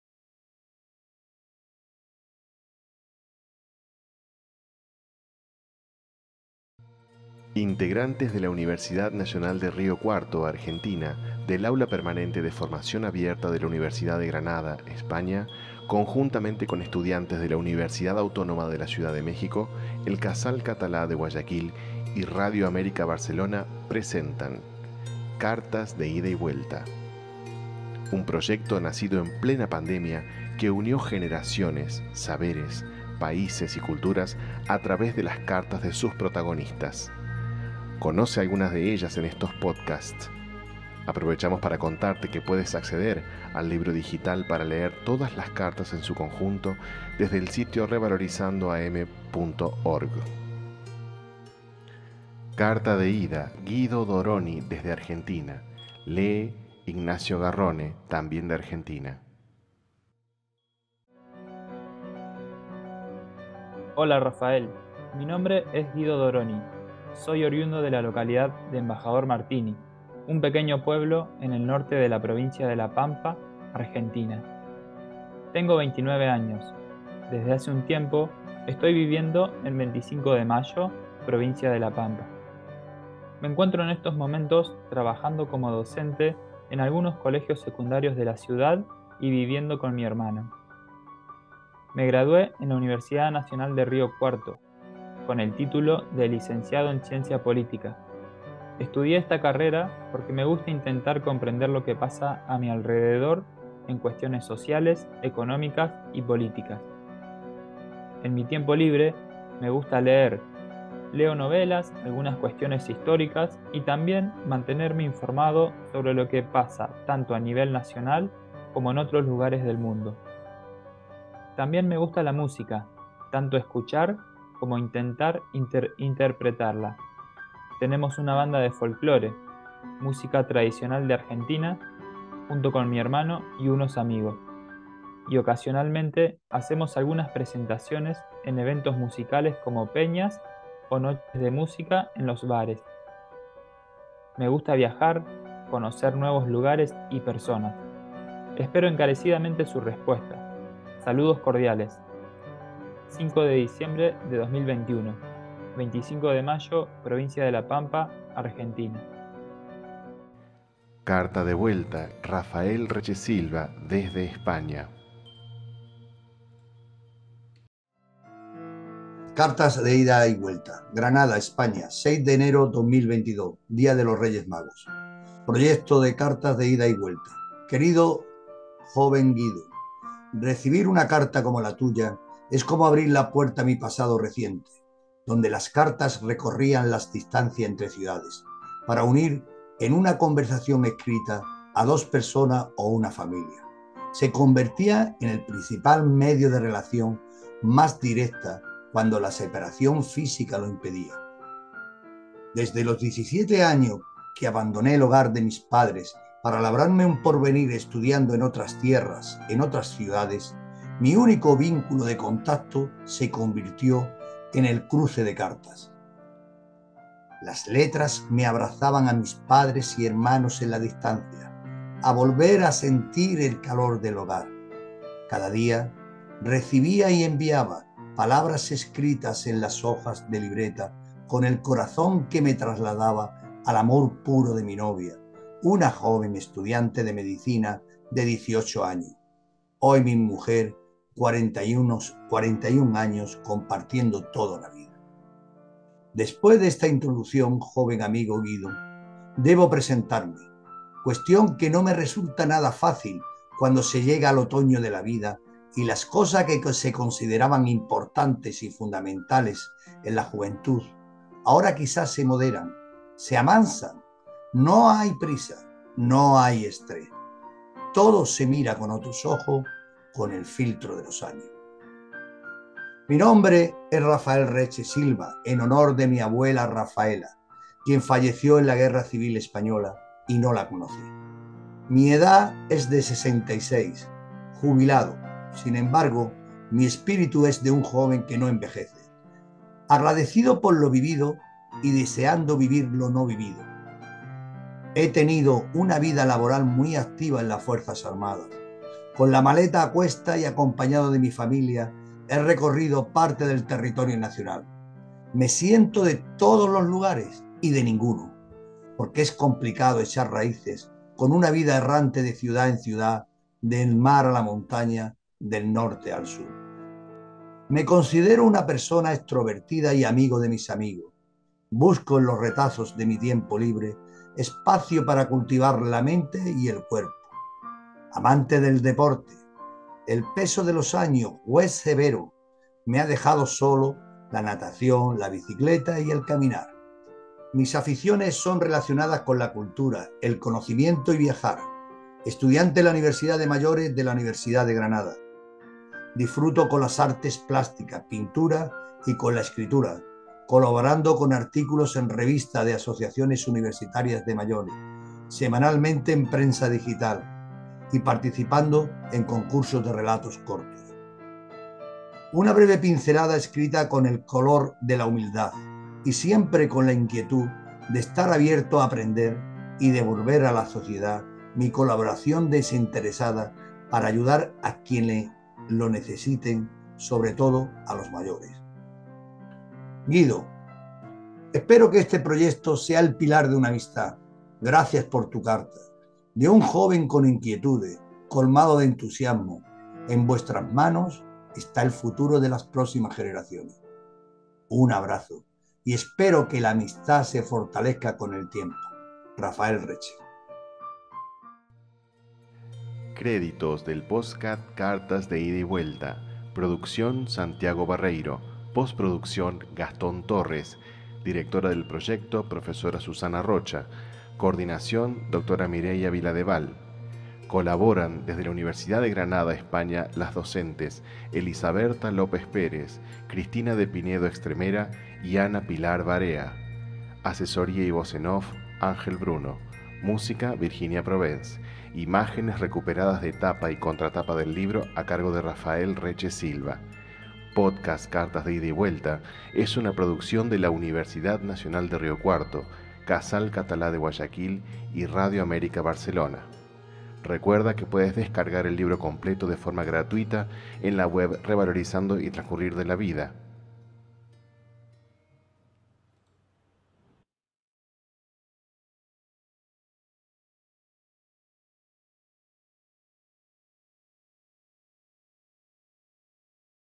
CARTAS DE IDA Y VUELTA - PODCAST 2
Integrantes de la Universidad Nacional de Río Cuarto, Argentina, del Aula Permanente de formación abierta de la Universidad de Granada, España, conjuntamente con docentes y estudiantes de la Universidad Autónoma de la Ciudad de México, el Casal Catalá de Guayaquil y RAB radio presentan:
"Cartas de Ida y Vuelta". Un proyecto nacido en plena pandemia que unió generaciones, saberes, países y culturas a través de las cartas de sus protagonistas. Conocé algunas de ellas en estos podcasts.

También te contamos que podés acceder al libro digital para leer todas las cartas desde el sitio: revalorizandoam.org
UniRio TV
Universidad Nacional de Río Cuarto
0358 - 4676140